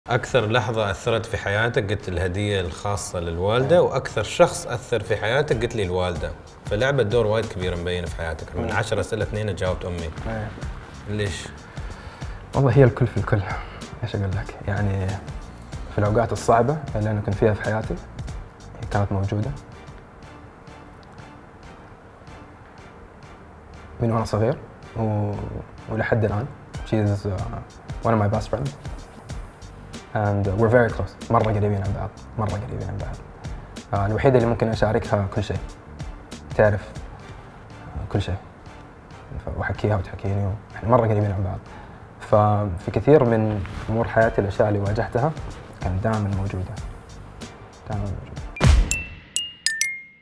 دور الوالدة - ملهم | ABtalks x NESCAFÉ
ما هي الدور الذي لعبت ام ملهم في حياته و كيف اثرت فيه؟

في هذا الفيديو الإضافي، يشاركنا ملهم مزيدًا من التفاصيل حول إجابته على أحد الأسئلة من حلقتنا على الإنستغرام؛ و من جوابه يعطينا صورة أعمق عن شخصيته.